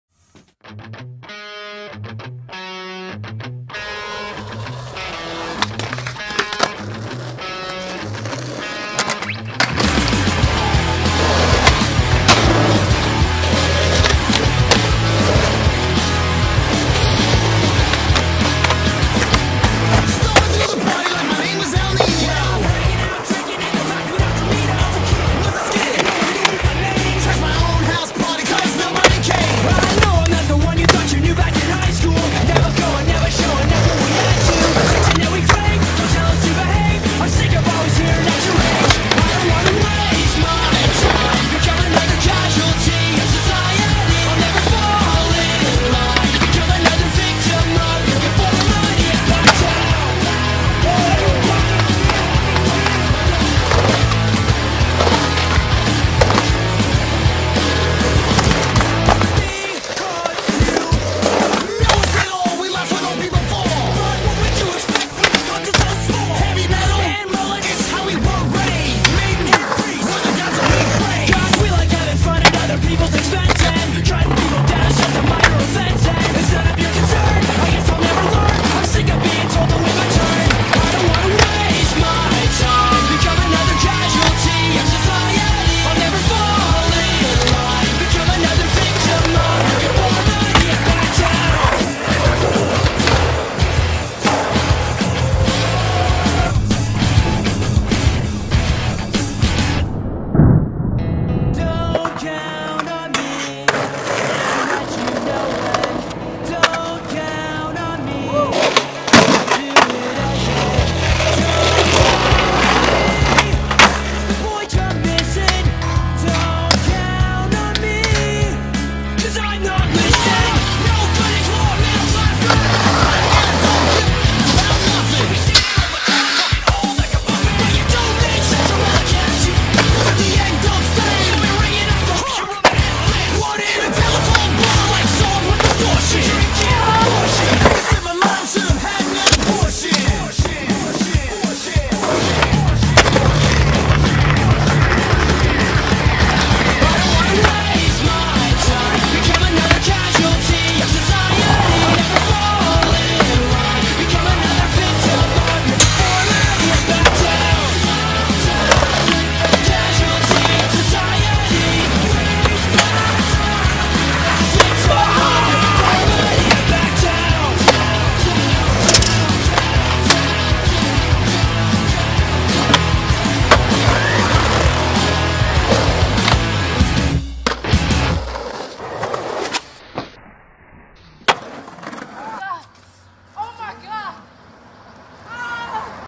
Chaswald.mov
best of foon 2k15, your girlfriends favorite skaters skating to her ex boyfriends favorite song
featuring the talents of Jzauss, Burnlord, lordfleek, jam pankus, Jacques Davis, Javris, Yitser, and Backwoodjonezzz